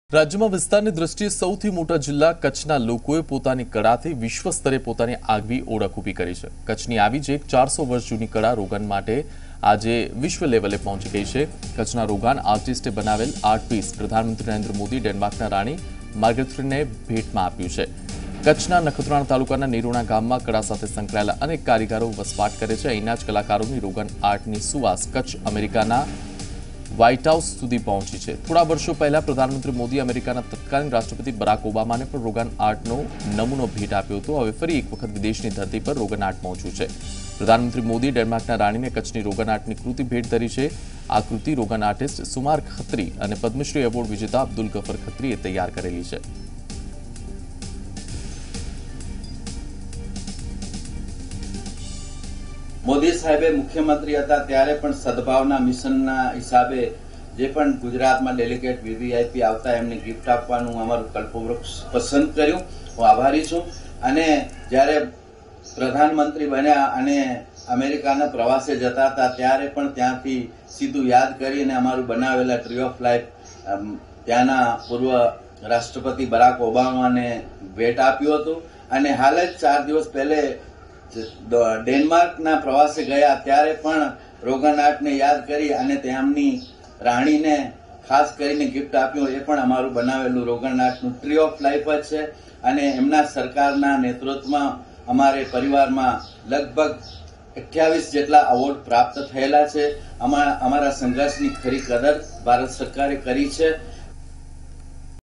Rogan Art 400-year-old Artist (Abdulgafur Khatri Padmashri) Nirona Village Kutch Gujarat india
If you need ccurate more information about Rogan Art Artist contact call

𝑻𝒓𝒂𝒅𝒊𝒕𝒊𝒐𝒏𝒂𝒍 𝑹𝒐𝒈𝒂𝒏 𝑨𝒓𝒕 (ABDULGAFUR KHATRI)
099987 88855